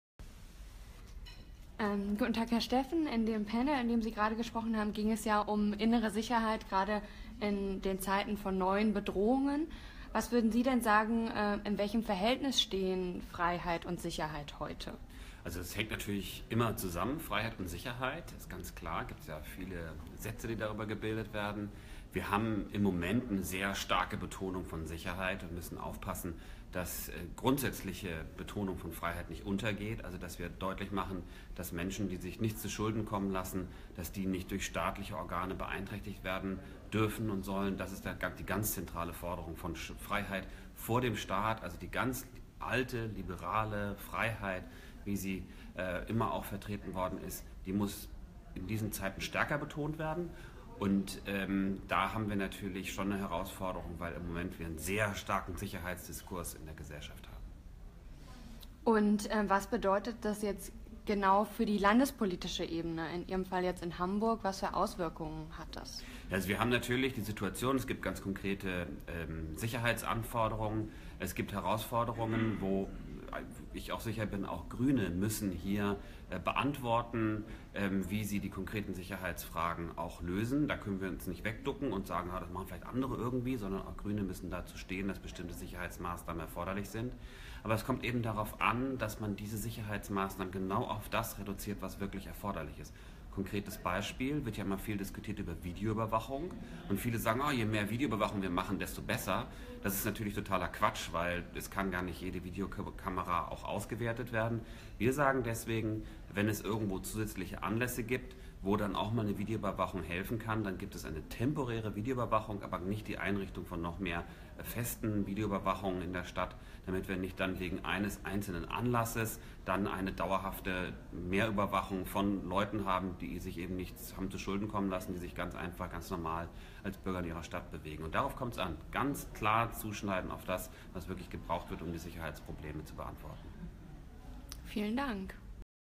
Was ist die Grüne Erzählung? Ein Interview mit Till Steffen.
Till Steffen, Hamburger Justizsenator, über innere Sicherheit auf der Konferenz "Die Grüne Erzählung".